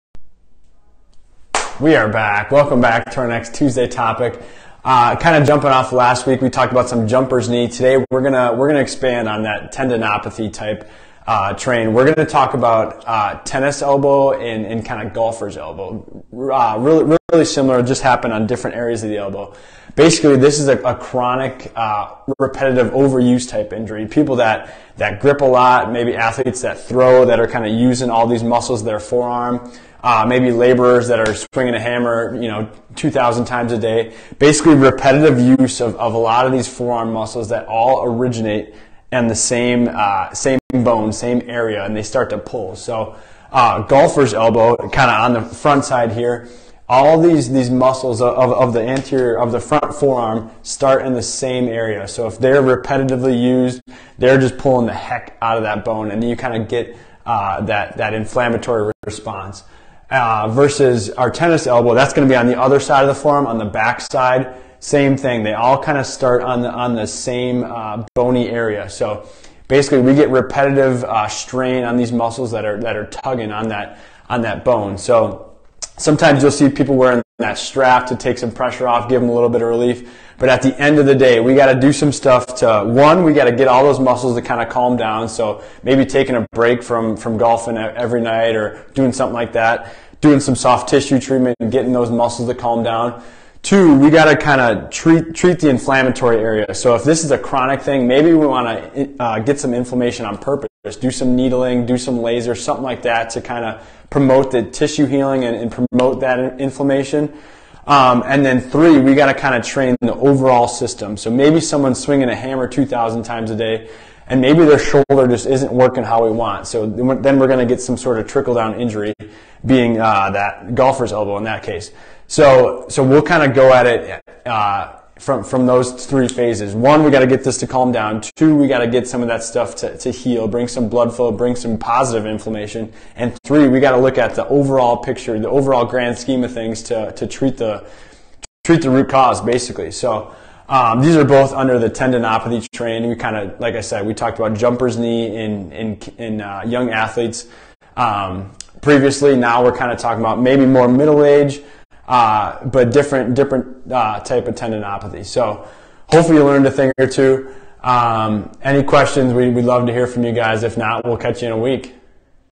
Tuesday Topic: Tennis and Golfer's Elbow
Dr. Peter Roy of Winchester Spine and Sport in Troy, MO discusses tennis and golfer's elbow.